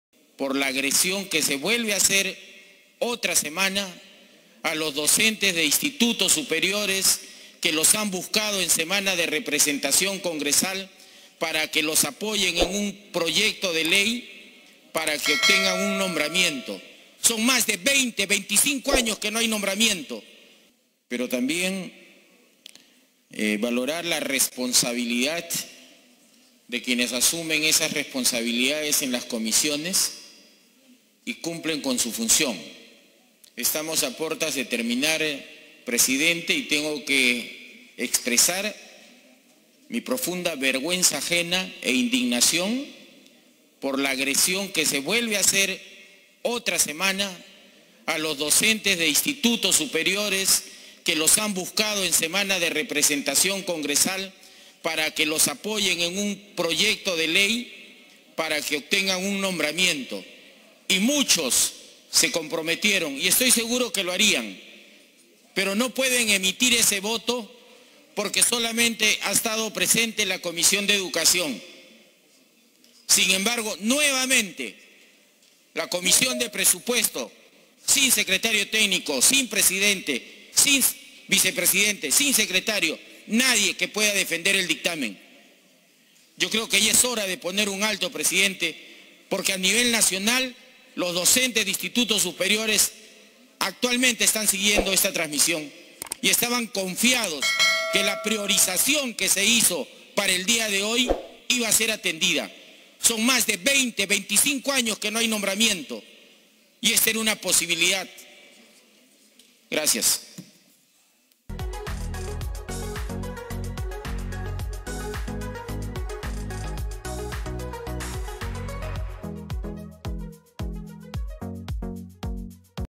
😱SE MOLESTÓ 👉ES UNA BURLA PARA LOS DOCENTES DE INSTITUTOS SUPERIORES MÁS DE 25 AÑOS SIN NOMBRAMIENTO
EL PROYECTO DE LEY …ES PARA  NOMBRAR A DOCENTES DE INSTITUTOS SUPERIORES